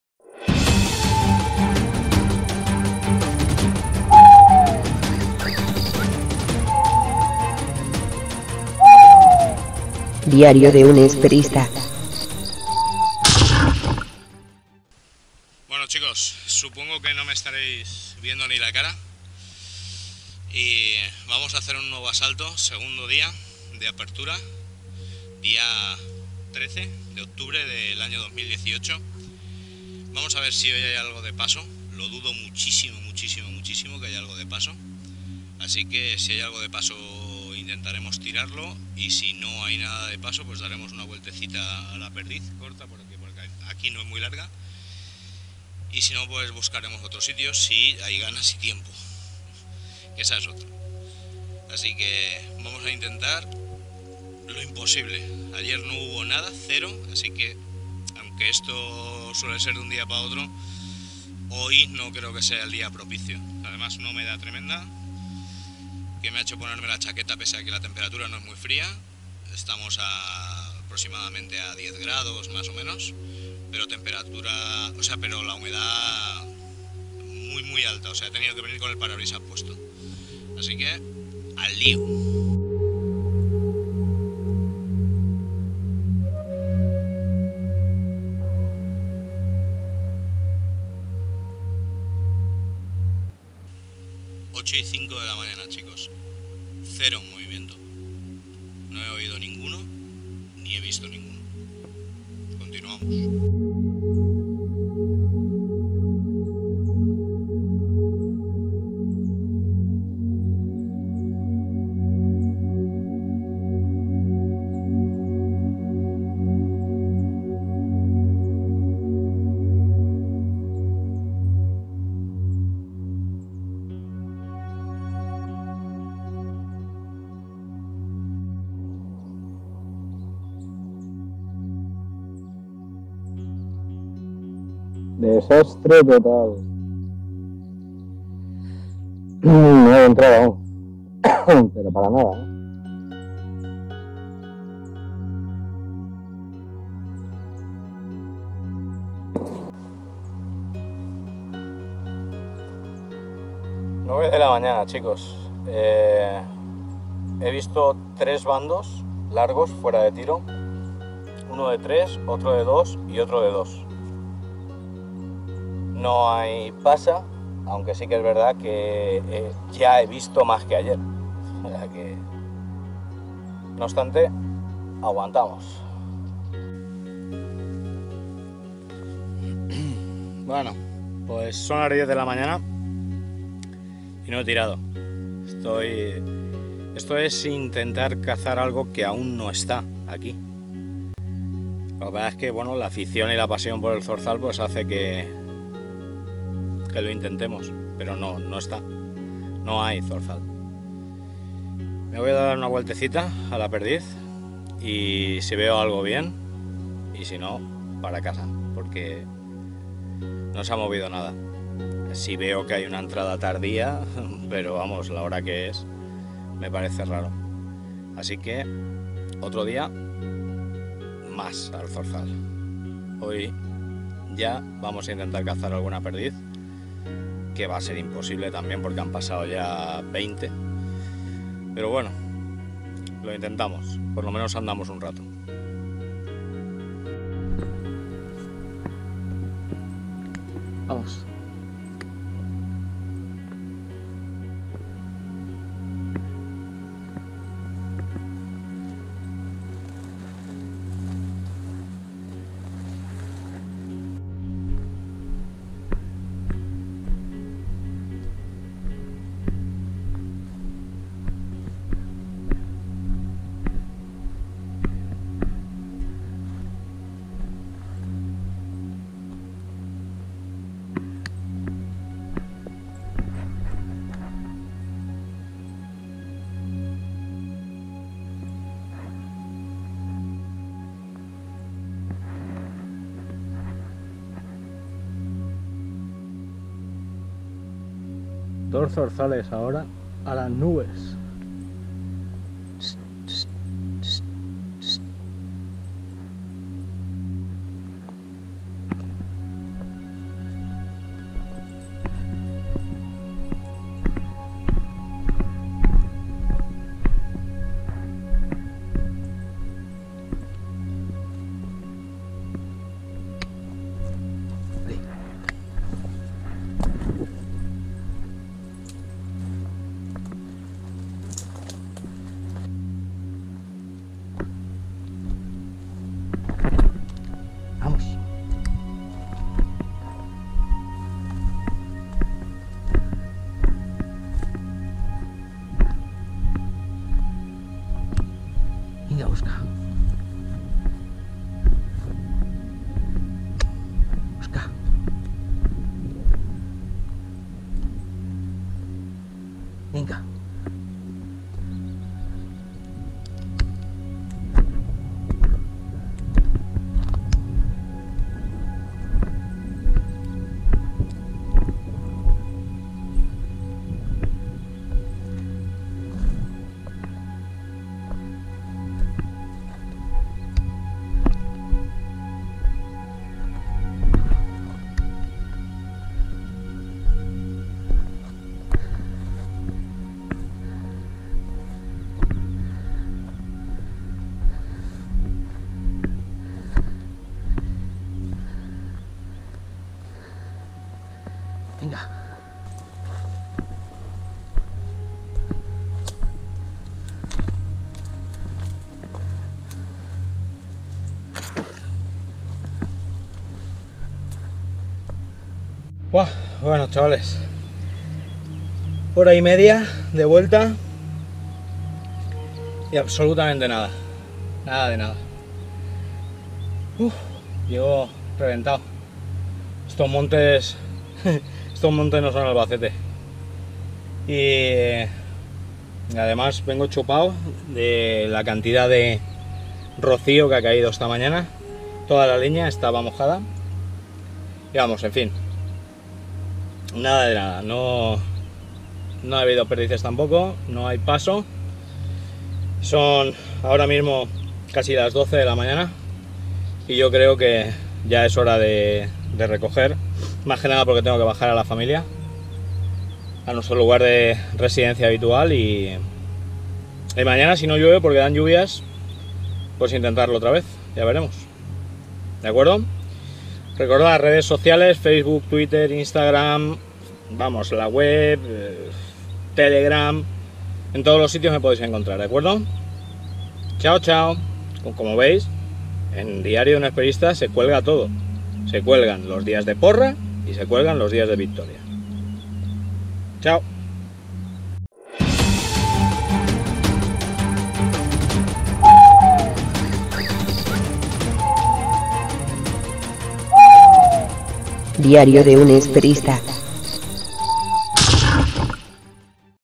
Caza. 13-10-2018
Probamos el segundo día de veda y lo hacemos en otra parte del coto más rodeada de pinar para poder ver si algunos animales entran y podemos pasar una buena mañana.